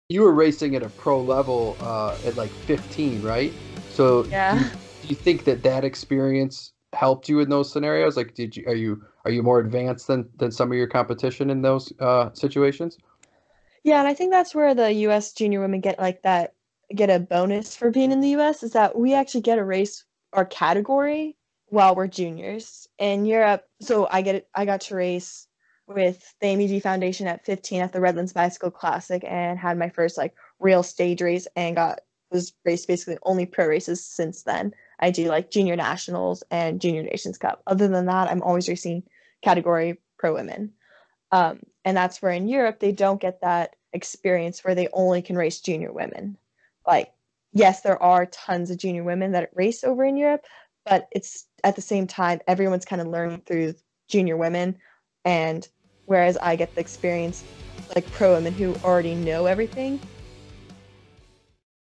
Episode #41 (Clip): Megan Jastrab on Racing Pro as a Junior.
Lining up against top pros since she was just 15, Megan Jastrab attributes her poise in junior competition to racing some of the best in the game . This clip highlights her ability to unleash a "late" sprint at Worlds, a sprint so nasty she still had time to post-up. But that's why she has three rainbow jerseys on her wall! Photos thanks to Rally UHC and Megan Jastrab. Full episode available here, on any podcast app or at packchatterpodcast.com.